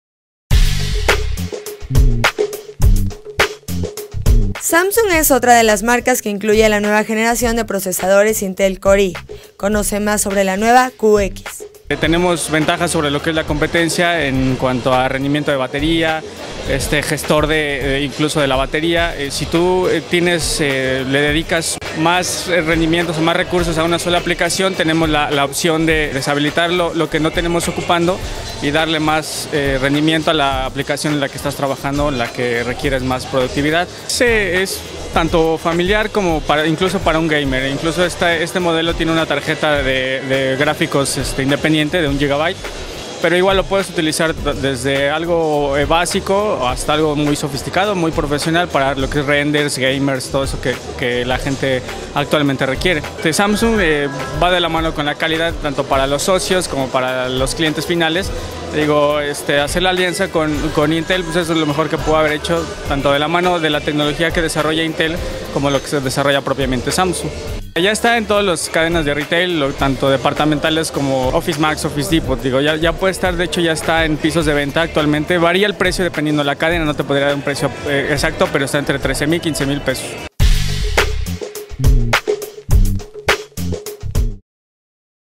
Samsung Serie QX con Intel Core i
La nueva computadora de Samsung incluye la segunda generación de procesadores Intel Core i.